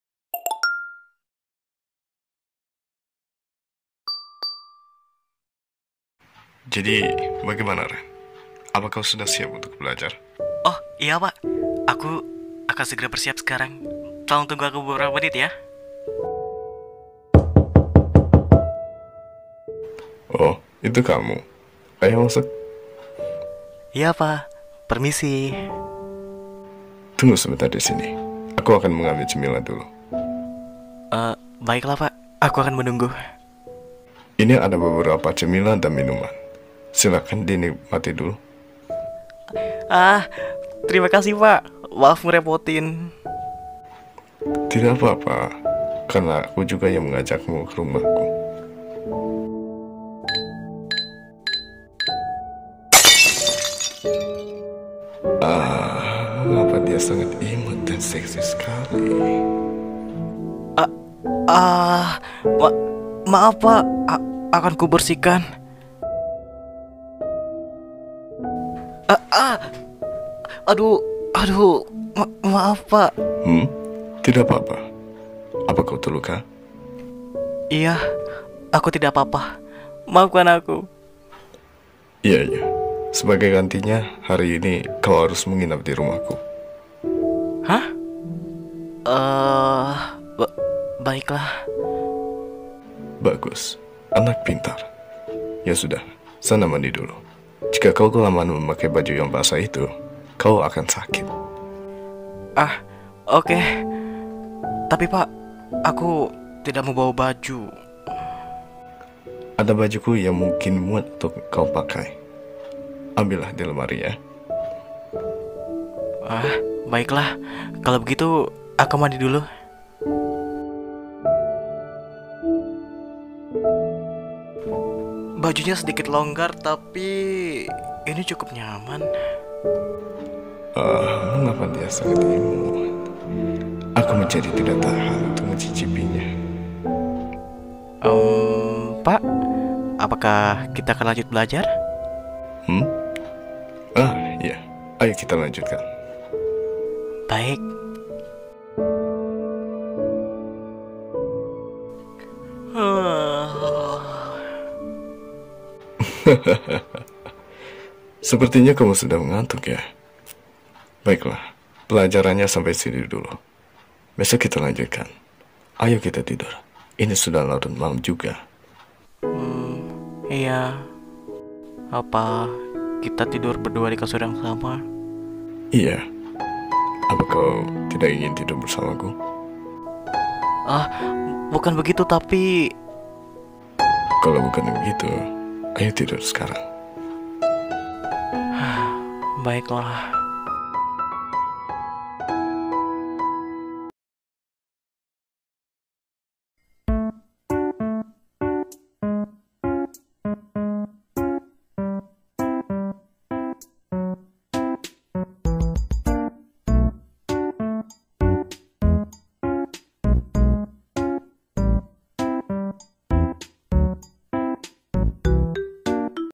BL VOICE ACTING | THE TEACHER STUDY DATE EPS. 2
Script by : Reina
Duet with Azura Gerry

Thank You uwu